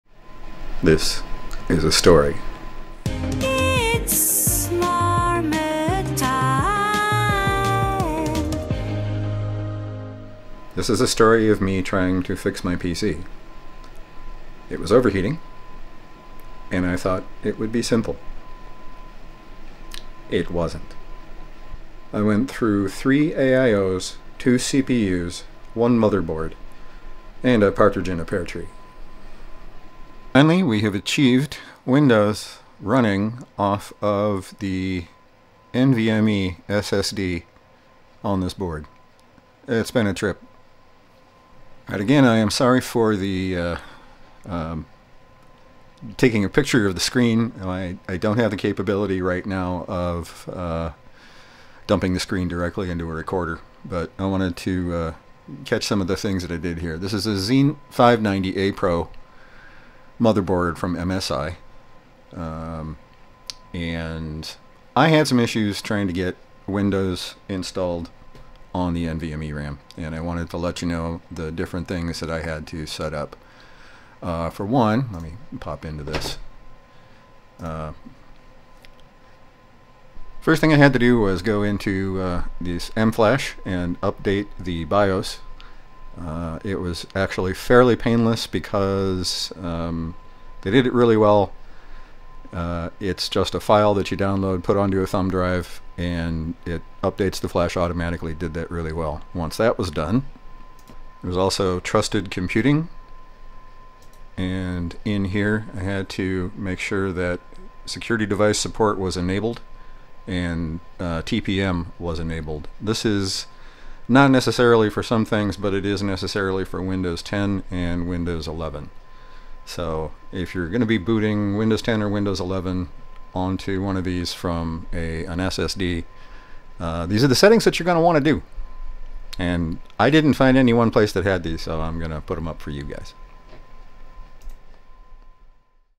Getting Windows to boot from NVMe on an MSI Z790A Pro motherboard
Yes, it's quite specific information, but it's information that it took me weeks to figure out.
First, to utilize the M2.1 slot, you need an 11th-generation or above CPU. It's an LGA1200 motherboard, so 10th-gen CPUs are available that work fine with the board but don't see the M2.1 slot. It has to do with the CPU architecture and there are other videos that explain it better than I could.
Second, I had to update the BIOS. This was a bit scary, but went smoothly.
Third, the Trusted Platform Module (TPM) needs to be enabled. For sure for Win11, it may be for Win10 as well.
Fourth, Secure boot must be enabled and set to Standard.

There may be other BIOS settings that a tweaker may know far better than I, but this got me able to rebuild my system after many weeks of downtime.